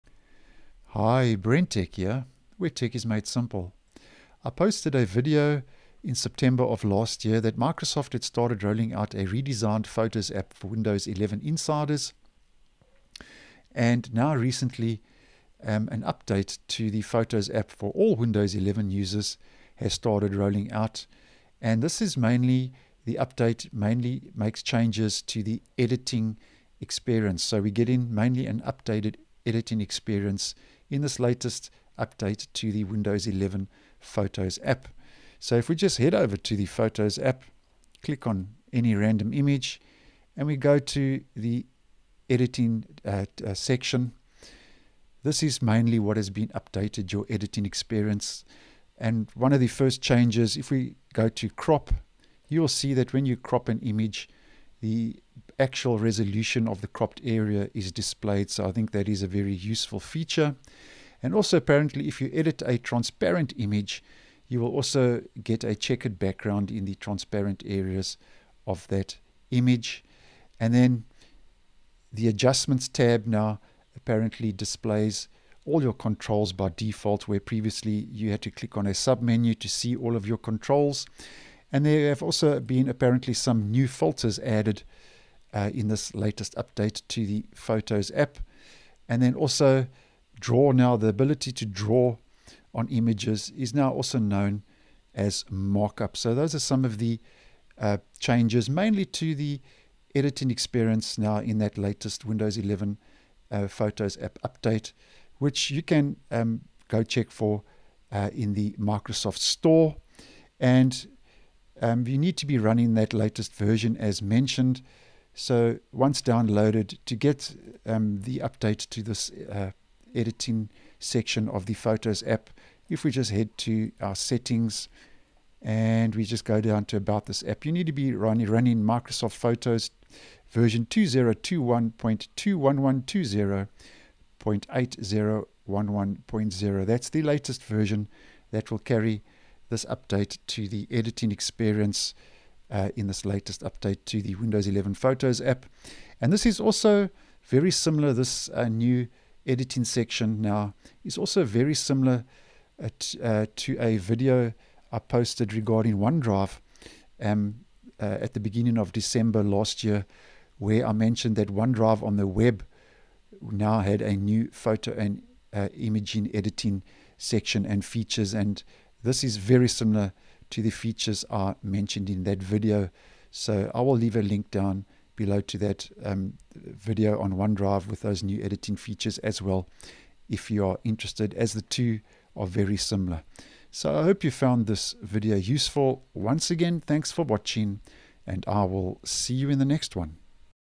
The Windows 11 Photos app now has updated photo & image editing features
Changes have been made to the Cropping tool, Adjustments tab, Filters and more.  OneDrive on the web also has very similar photo & image editing features